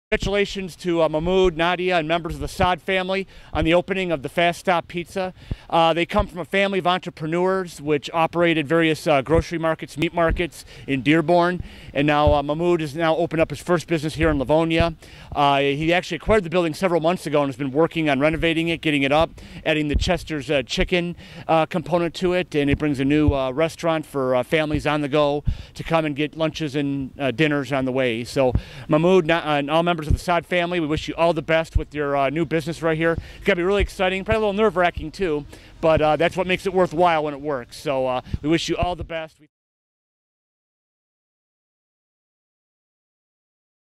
Fast Stop Pizza
Congratulations to Fast Stop Pizza on their grand opening. The ceremony took place on January 11, 2018 with the Livonia Chamber of Commerce and City of Livonia.